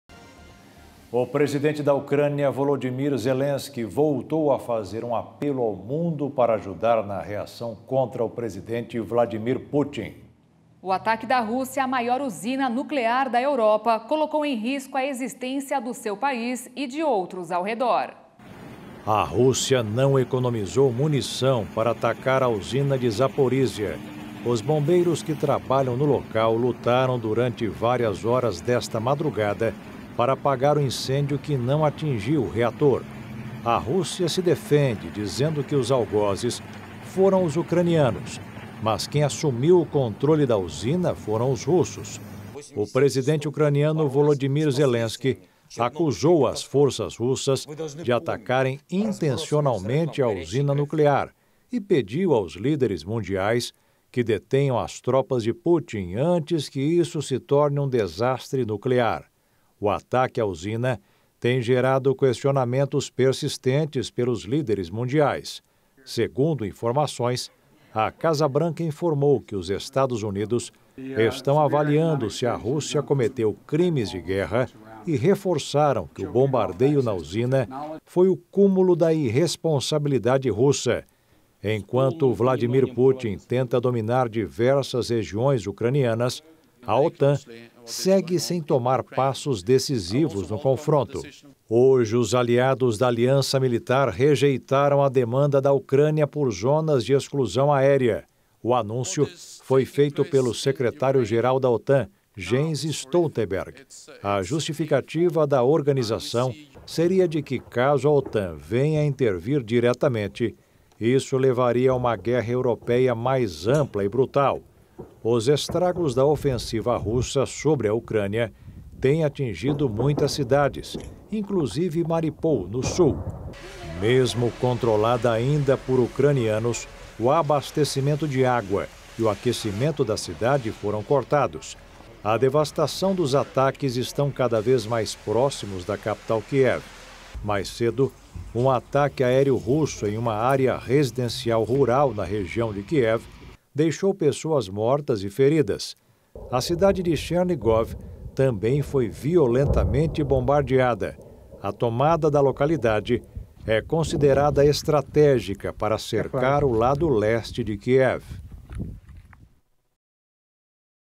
Maior usina nuclear da Europa está em chamas após ataque russo, diz autoridade
Um funcionário da maior usina nuclear da Europa no sudeste da Ucrânia disse nesta quinta-feira, 03, que tropas russas estavam bombardeando a instalação.